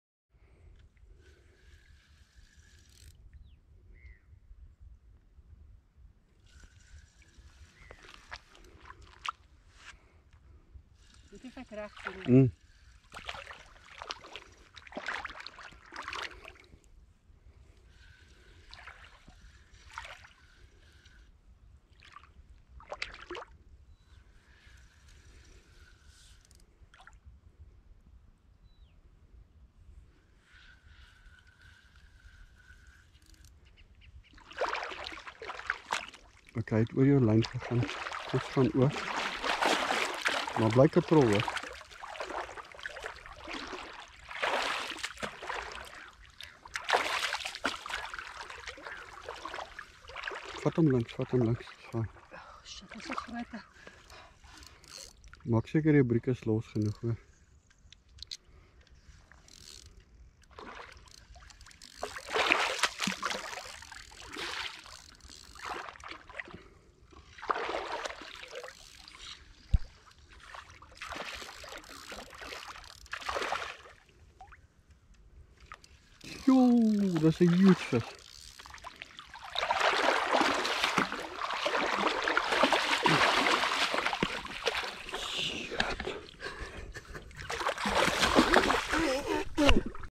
Carp action Darlington Dam
Michelle in action at Darlington Dam.